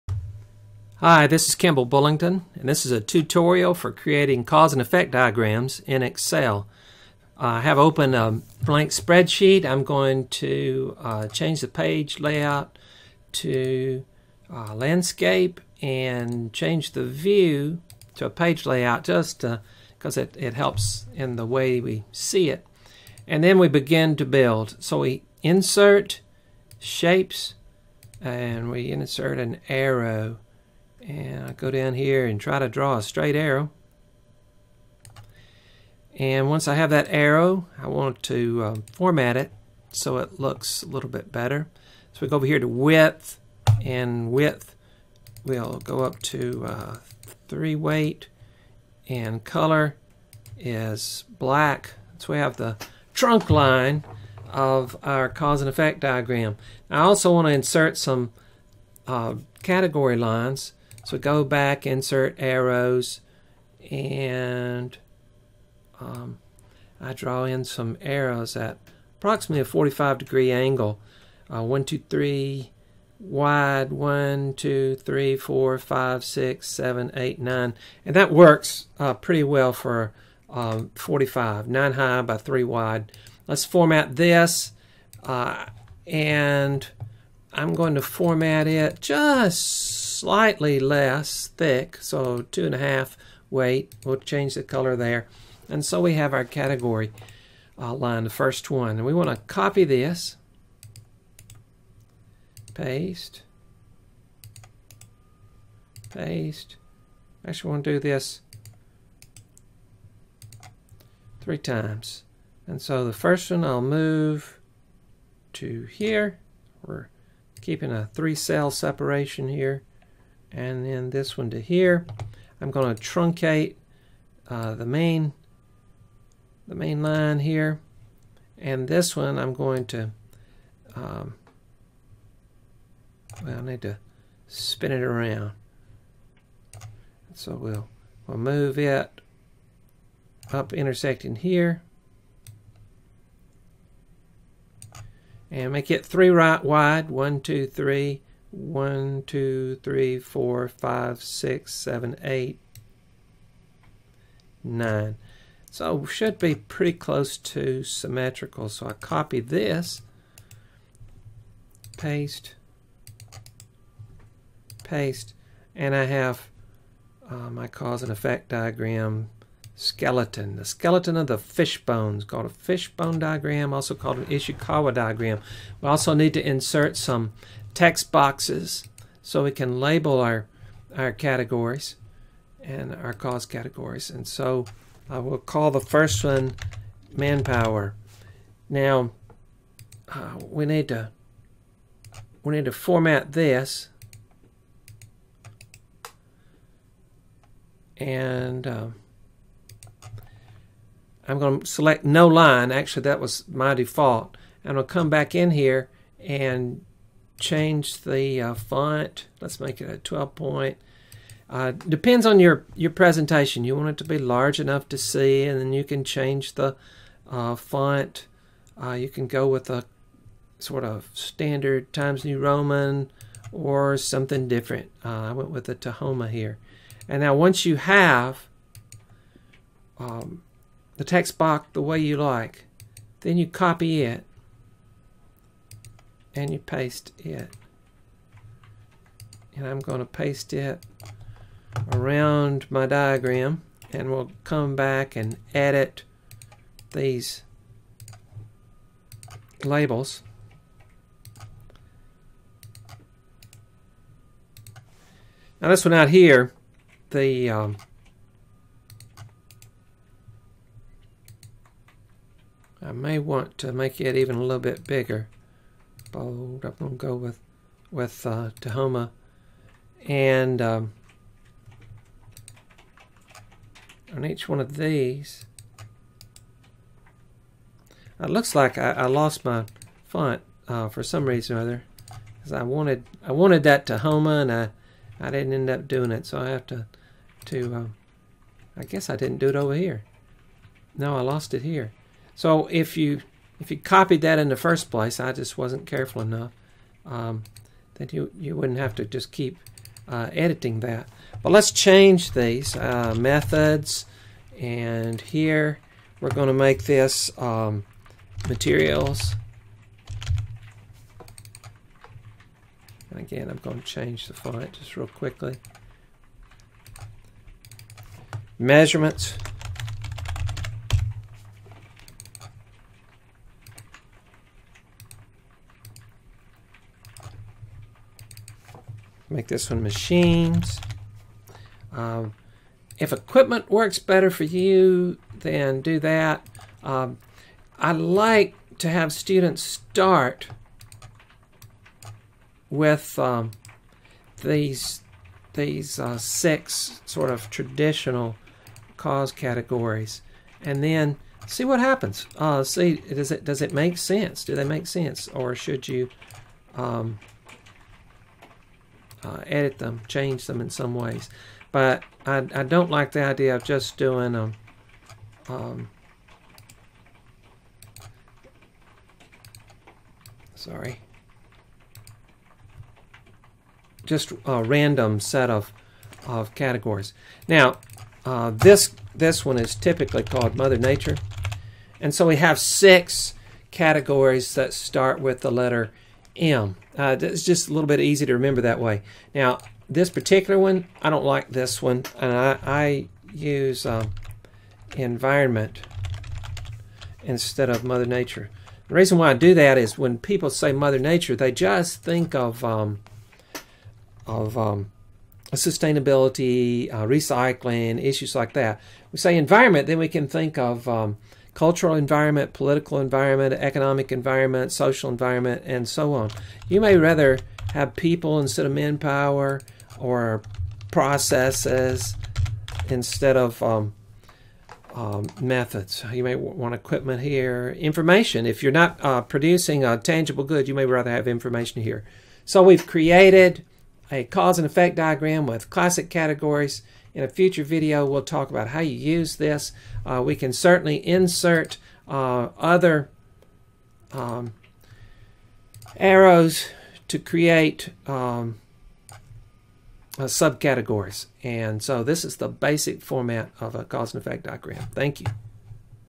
CED To Excel Tutorial - Part 1
Creating a basic cause-and-effect diagram using Excel.